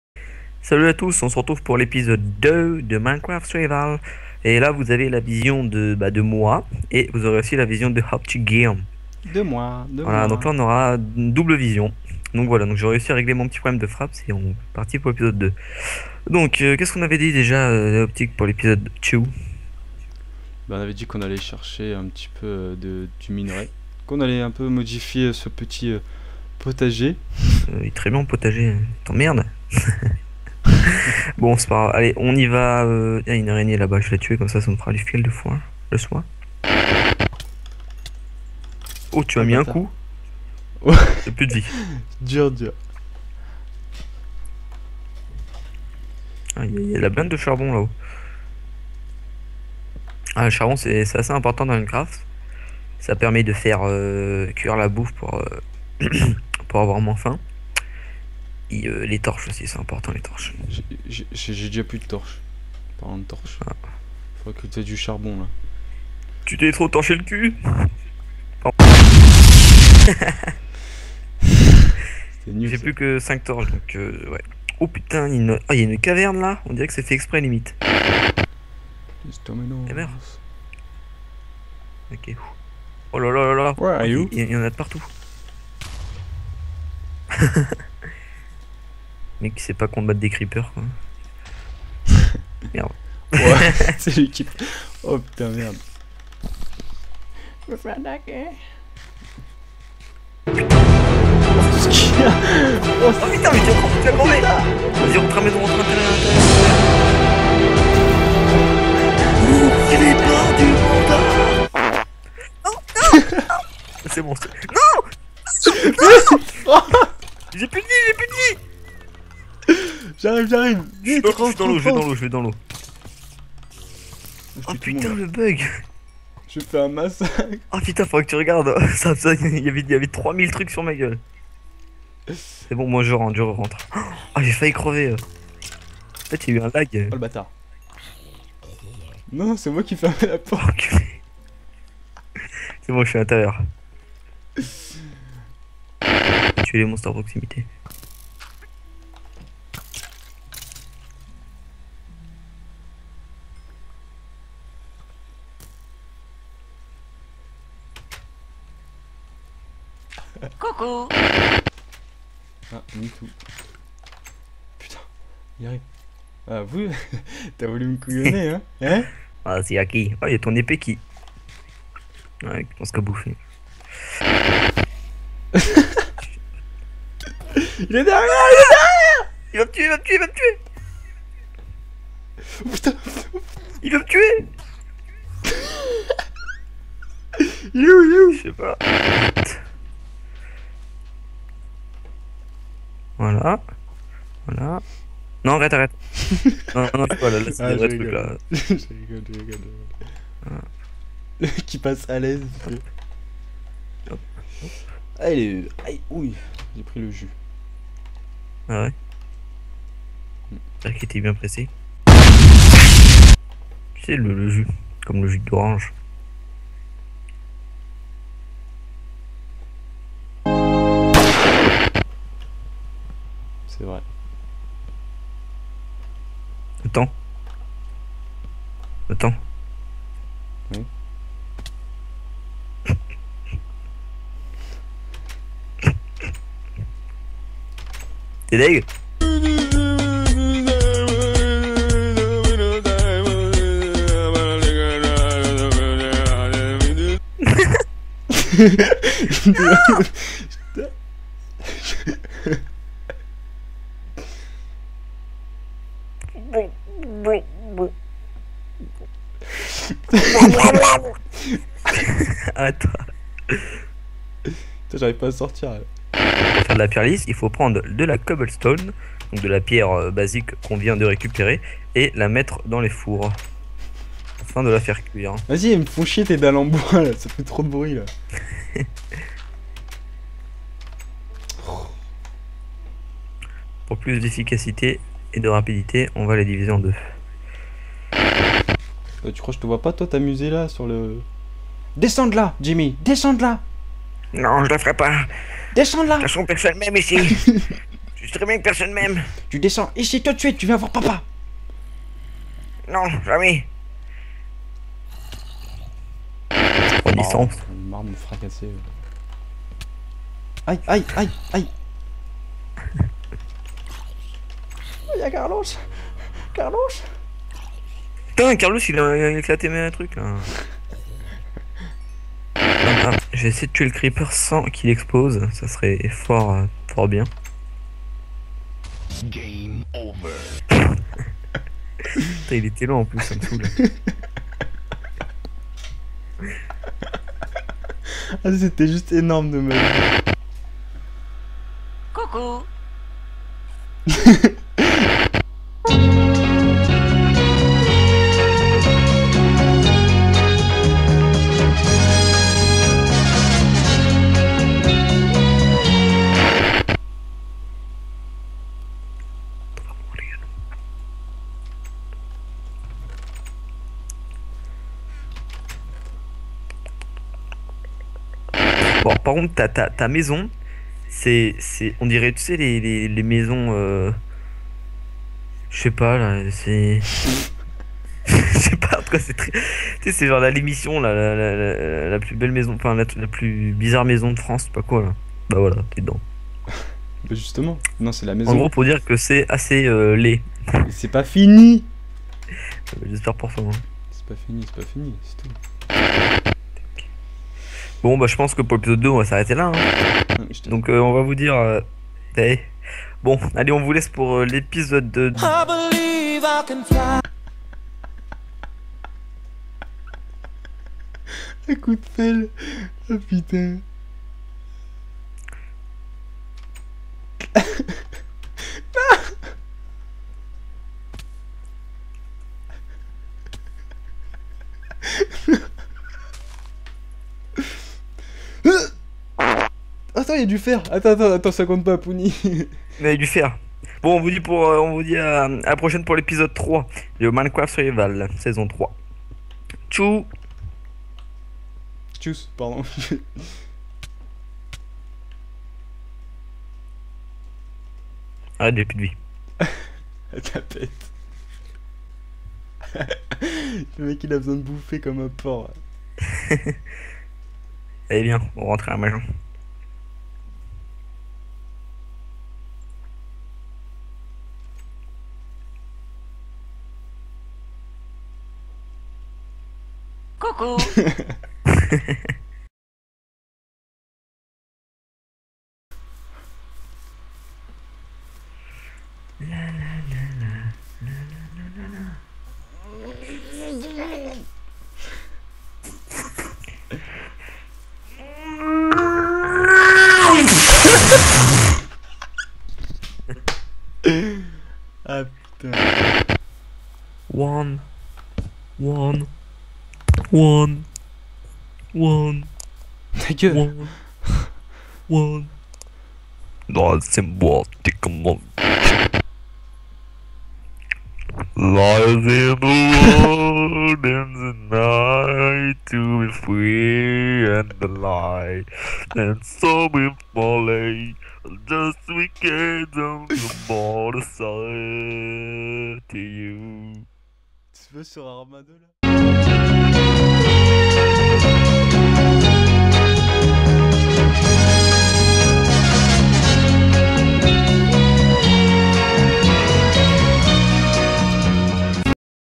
ZAP | Survival #2
Whath's up tous le monde,

Aujourd'hui c'est sur l'épisode n°2 qu'on se retrouve toujours en compagnie de mon collègue XStriaX, avec un montage toujours aussi foux !

Petit résumer de l'épisode 1 : Après avoir fait les présentation nous avons récolter du bois,suffisamment pour en construire notre petit chez nous, nous avons fais la rencontre d'un Pedo bear puis de deux Ender Man. XStriaX auras fabriquer sont potager qui me prendras toujours autant la tête et enfin une grande tragédie, nous avons commis un crime contre le peuple des cochons.

Voilà chousss et bonne vidéo ;)